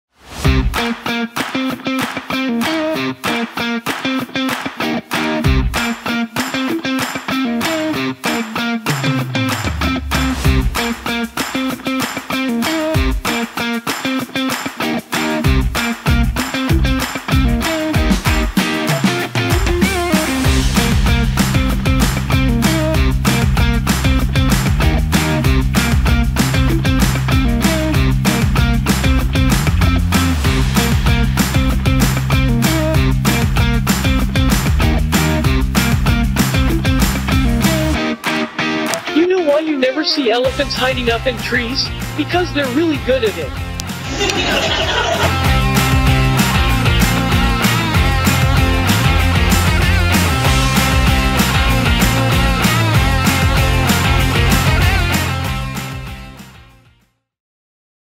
Spencer - build and code your own voice assistant
Spencer is a DIY voice assistant that talks, lights-up, connects to the internet, and understands simple voice commands.
He also has a funny personality, tells jokes, and finds new ones online.